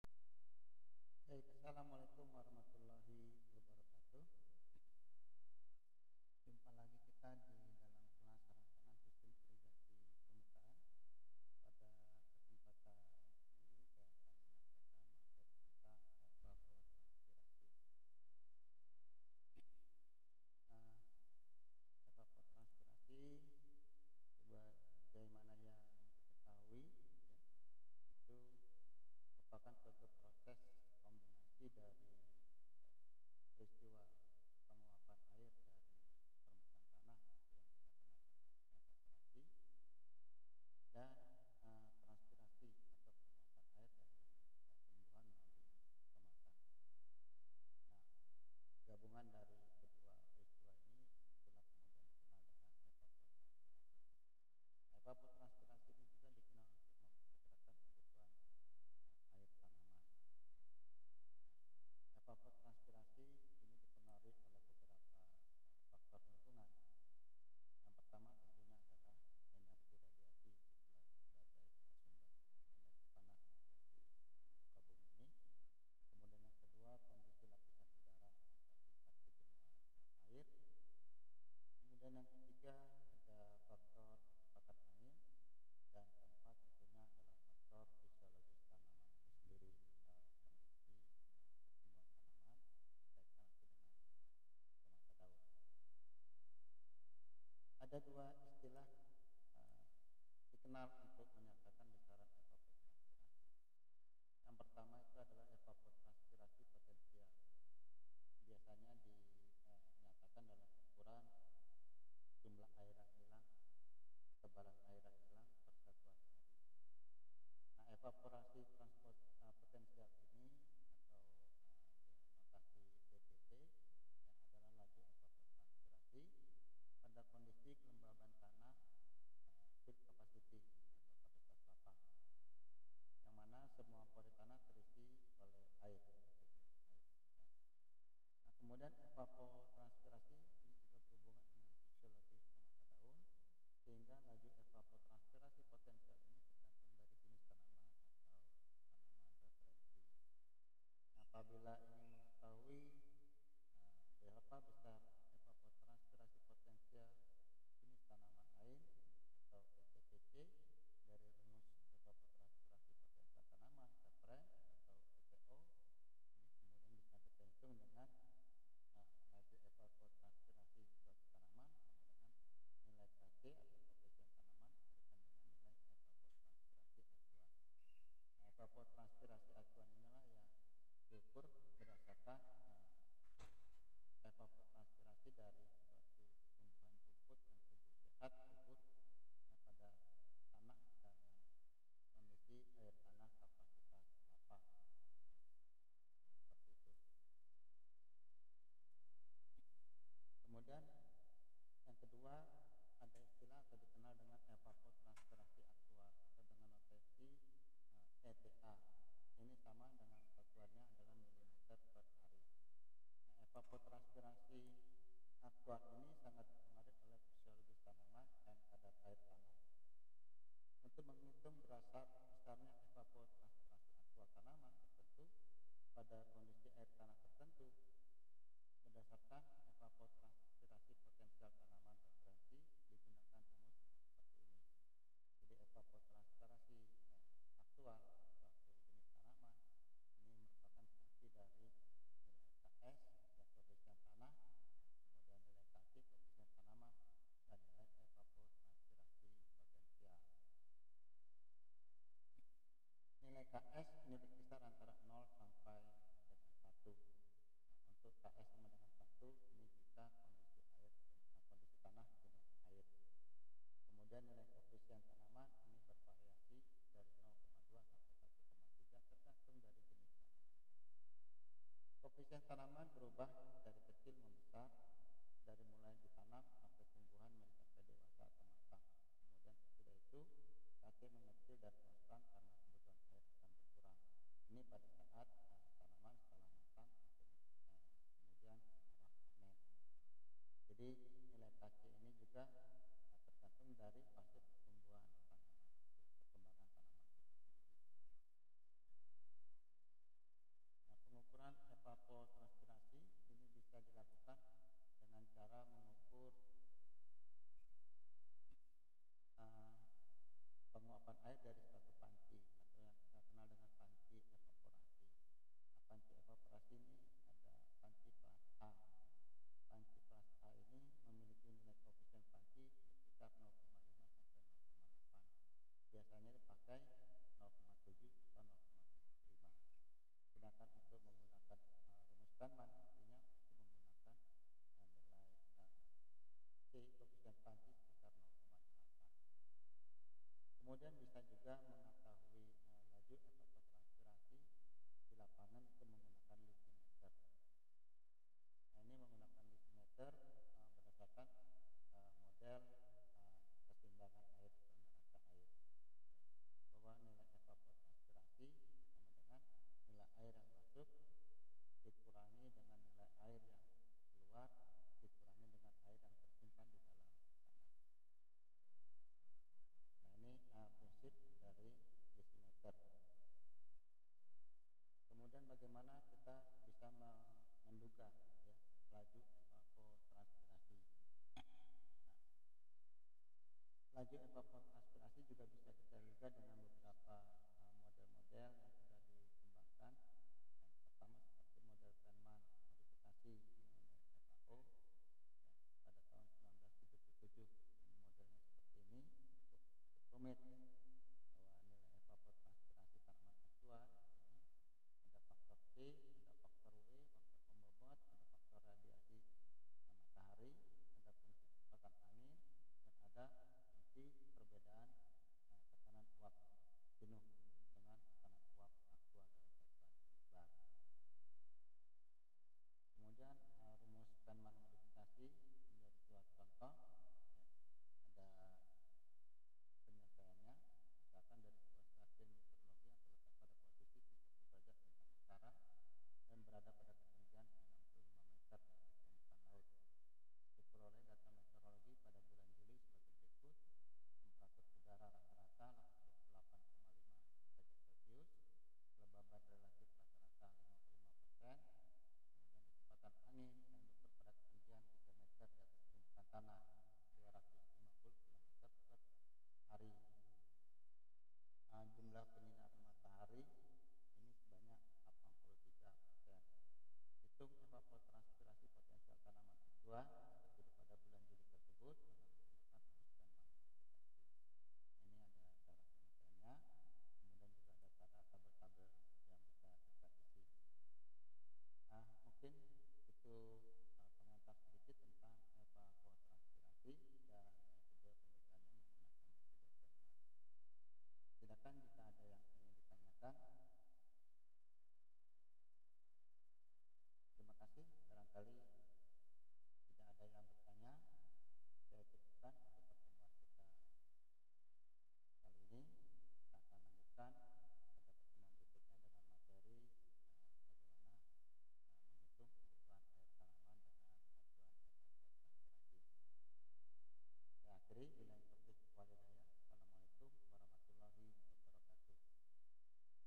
06. TEP - Perancangan Sistem Irigasi Permukaan - Evapotranspirasi
Pokok Bahasan : Evapotranspirasi
Dosen : Dr. Ir. Ridwan, M.S.,

Deskripsi :
Mata kuliah ini mempelajari tentang Rancangan sistem irigasi dan drainase permukaan, berisi tentang tata letak jaringan irigasi/ drainase permukaan (preliminary lay-out) di jaringan utama, penentuan elevasi muka air rencana, penampang memanjang dan melintang trase saluran, dan rancangan detil petak tersier. Rancangan sistem irigasi bertekanan mengenai pengertian, sistem dan jaringan irigasi curah dan tetes, serta perencanaan dan operasi sistem irigasi curah dan tetes.